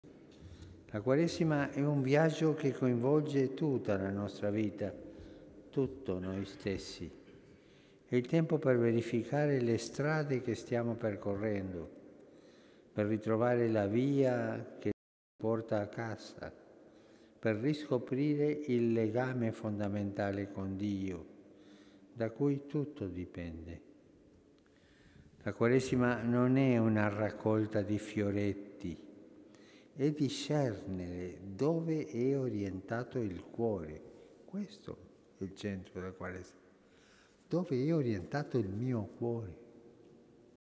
Papa Francesco Messa Mercoledì delle Ceneri 2021-02-17
Alla Messa del Mercoledì delle Ceneri celebrata nella Basilica di San Pietro, Francesco esorta a farsi piccoli intraprendendo un cammino di umiltà che porta alla Pasqua. Il Papa ricorda che "la salvezza non è una scalata per la gloria, ma un abbassamento per amore"